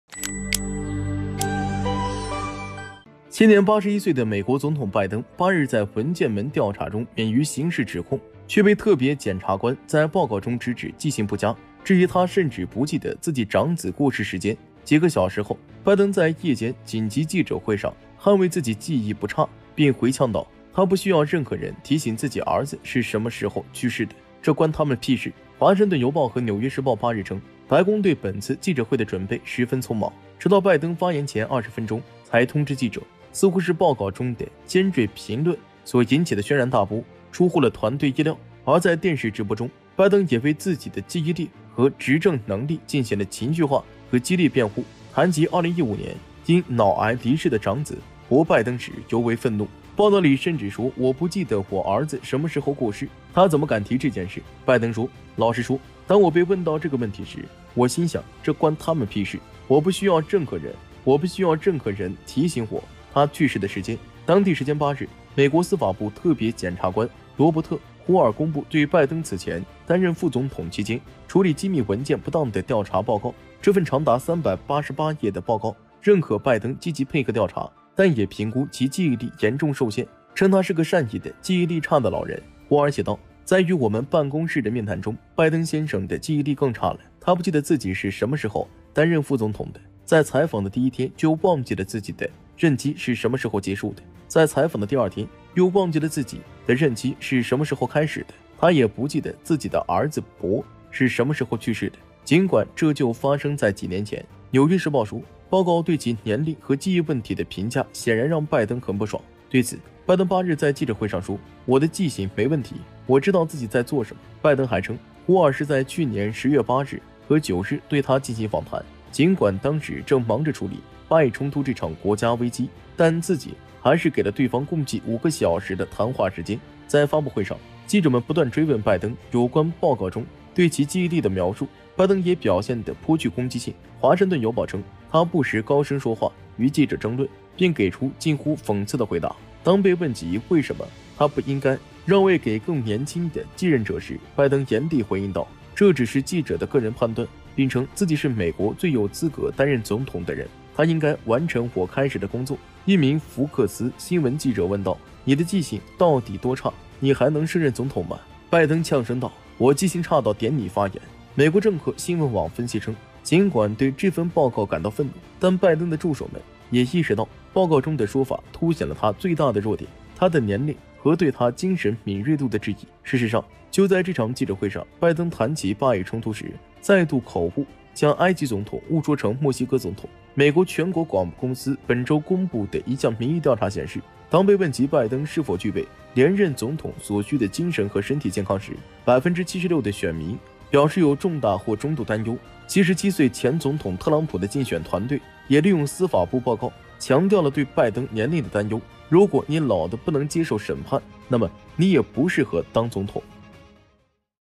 遭质疑“连儿子过世时间都不记得”，拜登怒了：“关他们屁事？”
现年81岁的美国总统拜登8日在“文件门”调查中免于刑事指控，却被特别检察官在报告中直指“记性不佳”，质疑他甚至不记得自己长子过世时间。几个小时后，拜登在夜间紧急记者会上捍卫自己记忆不差，并回呛道，他不需要任何人提醒自己儿子是什么时候去世的，“这关他们屁事？”